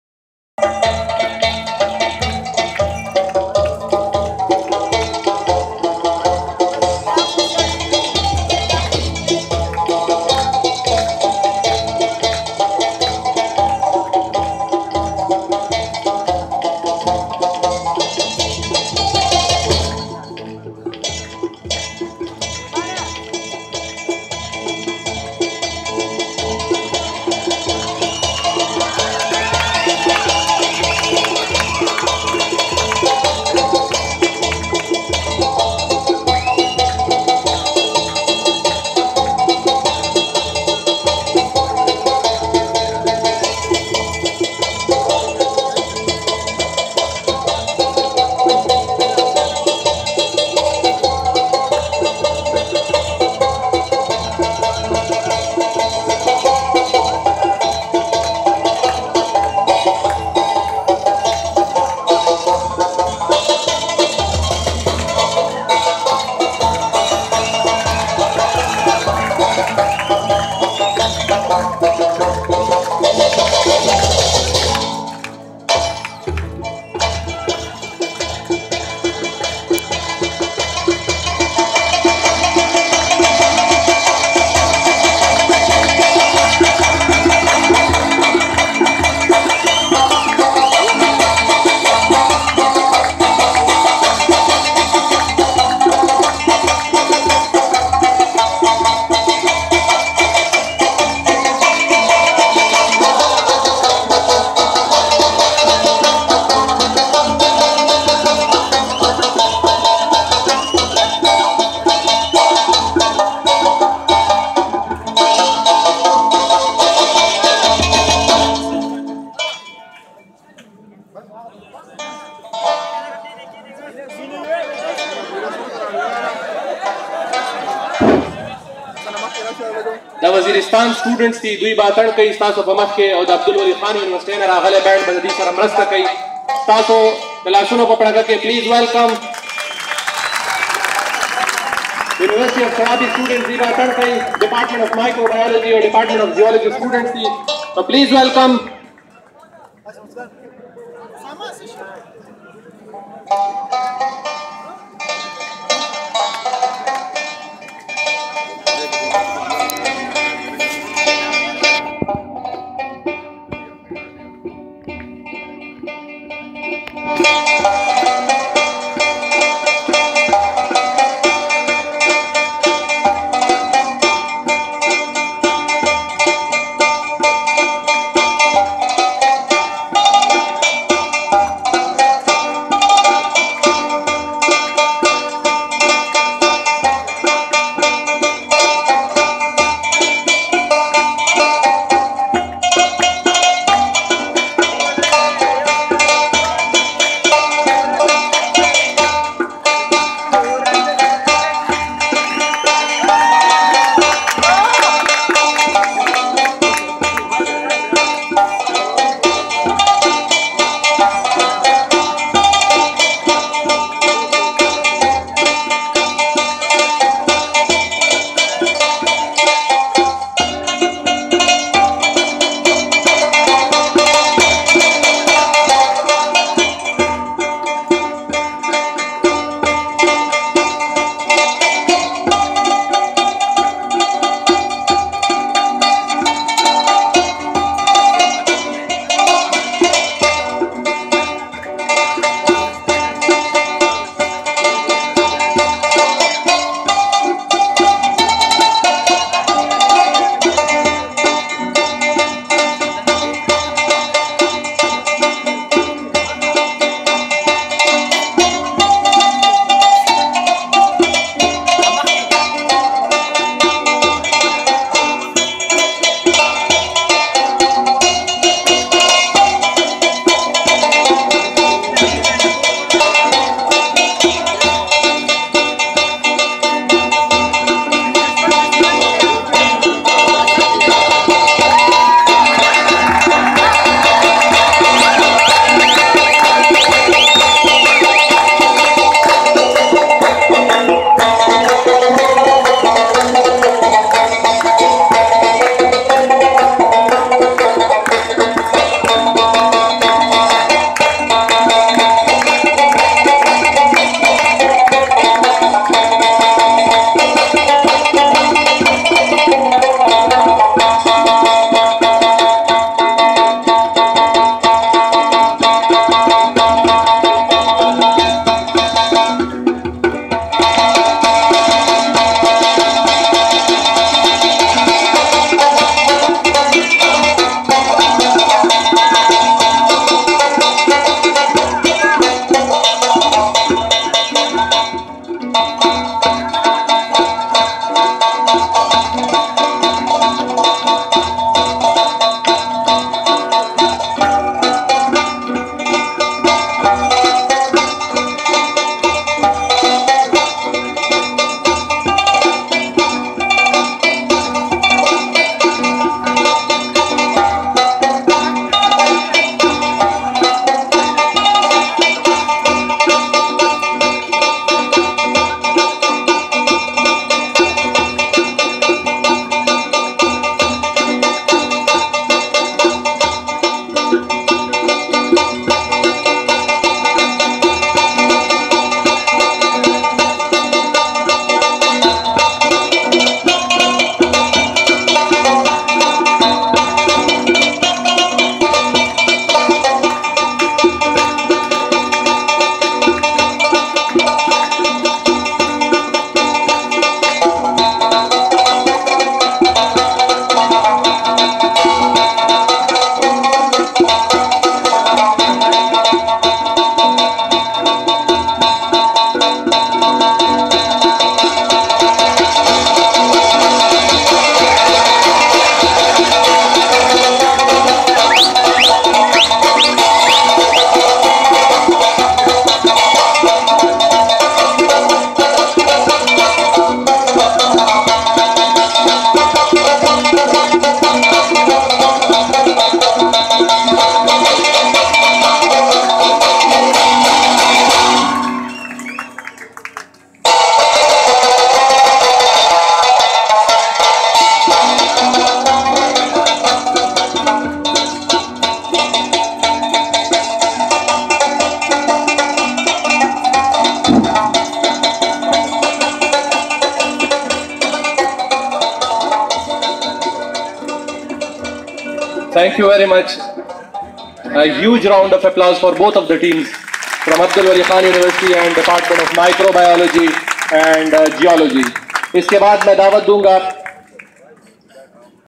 5th annual foundation day of university of Swabi
Pushtuns type of beautiful Rabab and Tabla. listen and enjoy